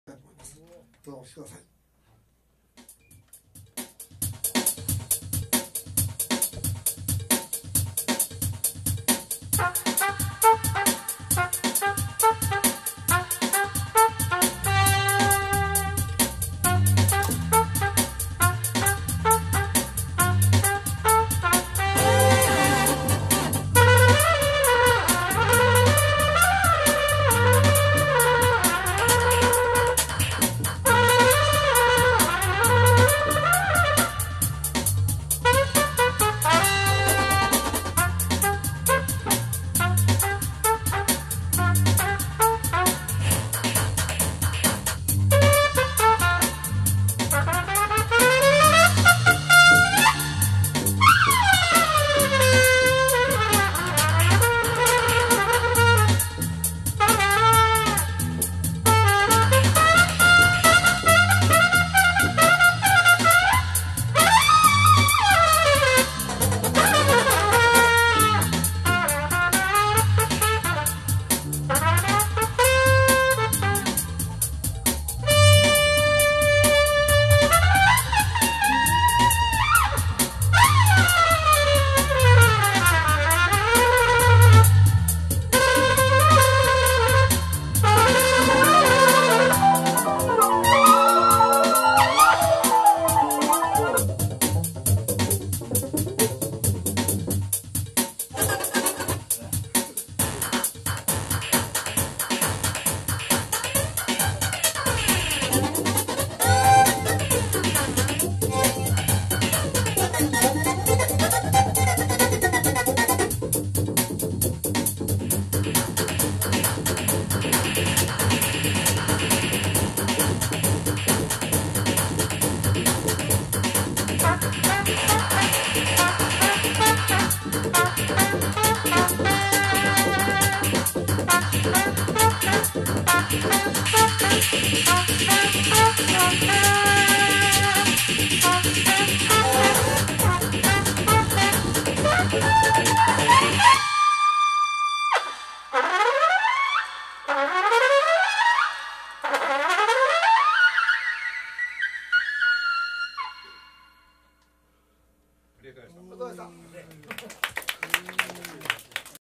アガルタ① HOTLINE2016 島村楽器鹿児島店 店予選動画
2016年7月3日に島村楽器鹿児島店で開催されたHOTLINE2016 店予選のアガルタさんのライブ映像です。

「HOTLINE」は、島村楽器が主催する、全国規模のコンテストイベントです。